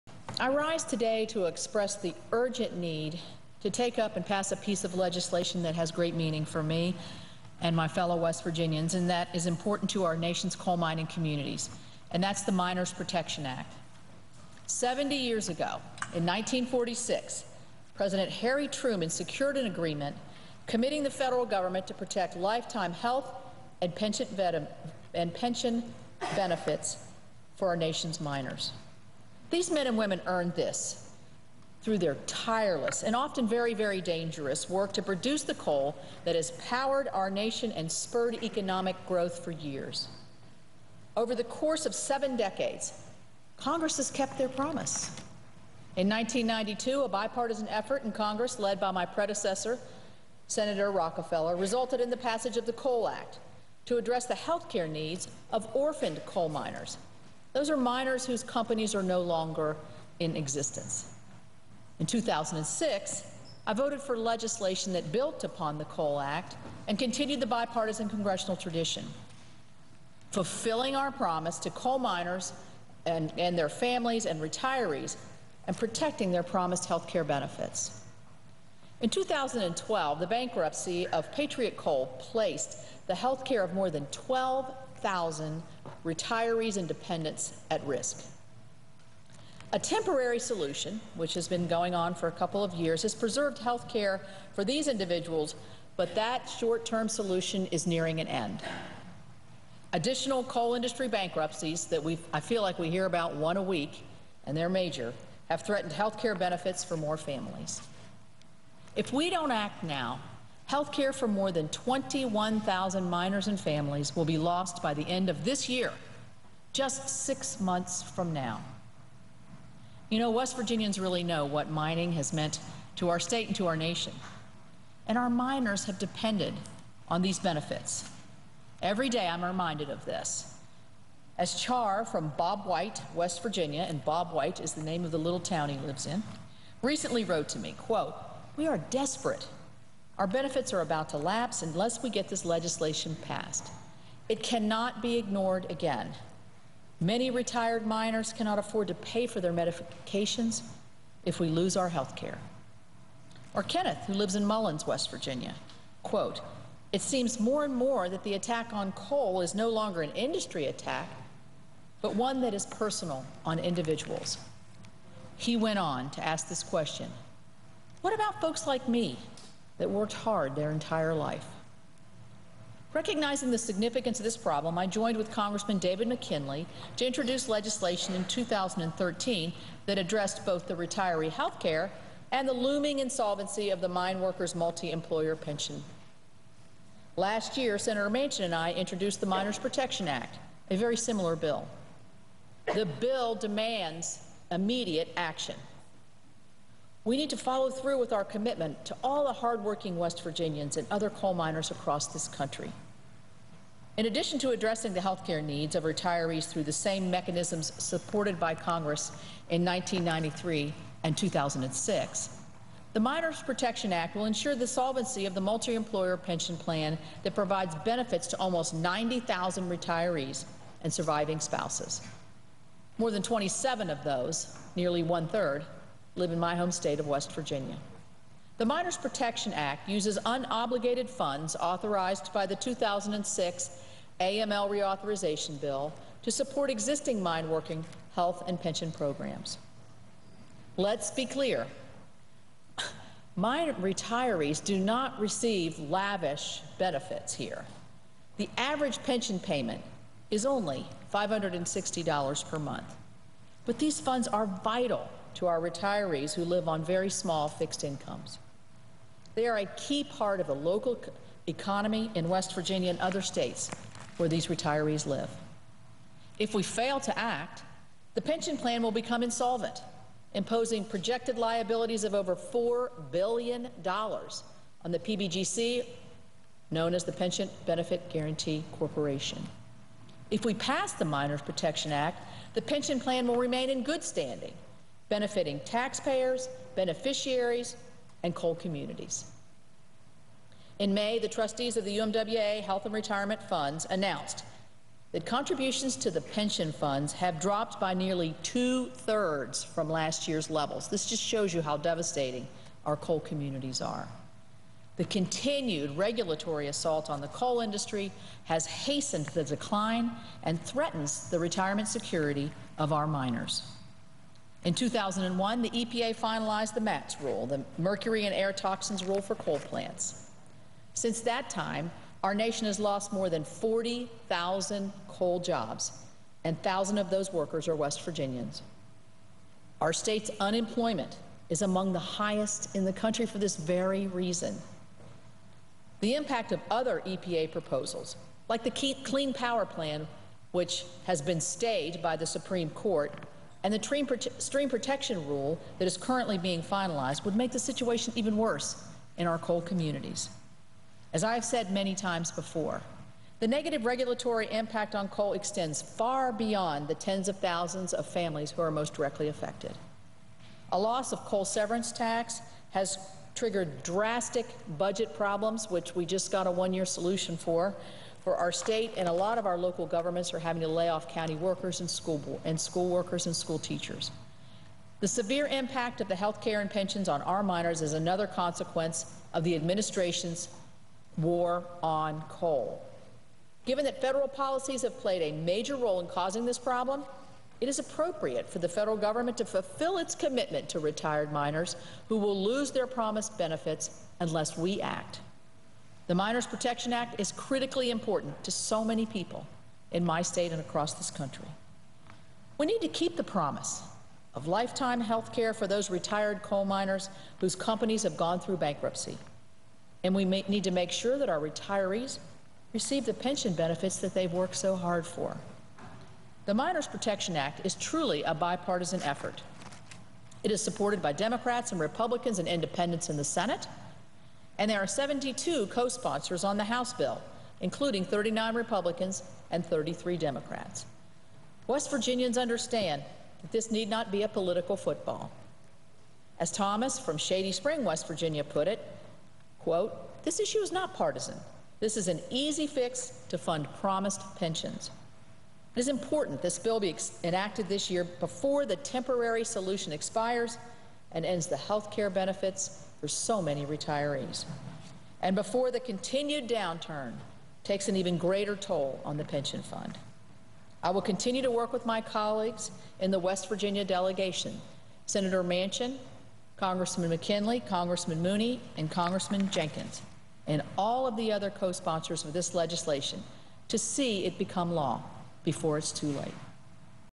U.S. Senator Capito speaks in support of Miners’ Protection Act
U.S. Senator Shelley Moore Capito (R-WV) spoke during the June 21st floor proceedings about the importance of the Miners’ Protection Act, S.1714. Thank you for your support Senator Capito! Go to umwa.org to learn more about what you can do to pressure Congress to pass this critical legislation, which has life or death implications for tens of thousands senior citizens across America.